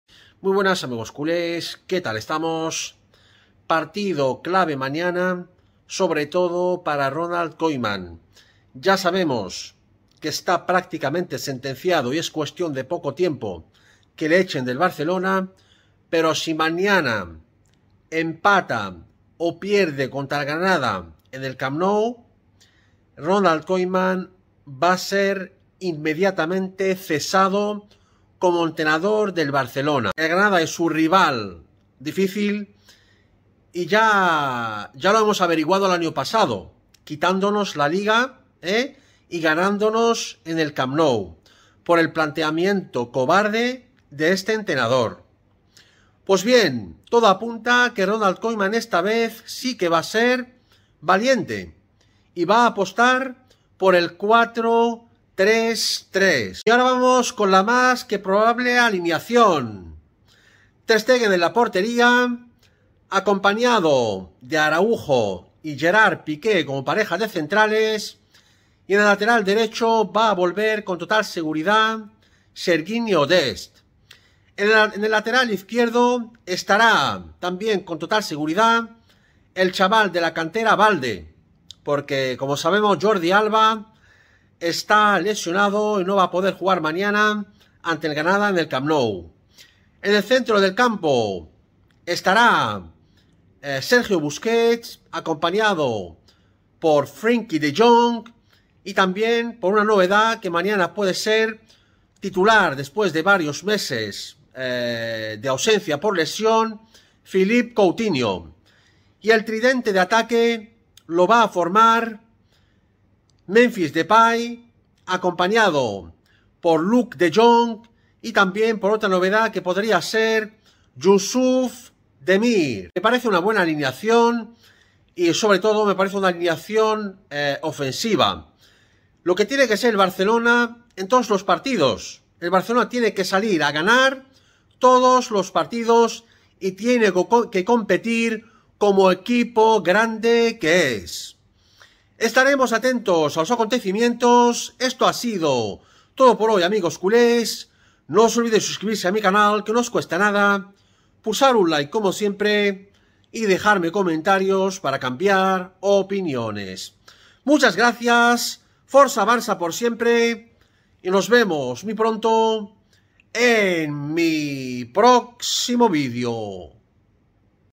¡¡LA ÚLTIMA LOCURA DE ALINEACIÓN DE KOEMAN CONTRA EL GRANADA, BARCELONA VS GRANADA!!
Koeman se la juega mañana lunes en el Camp Nou contra el Granada con una alineación de locura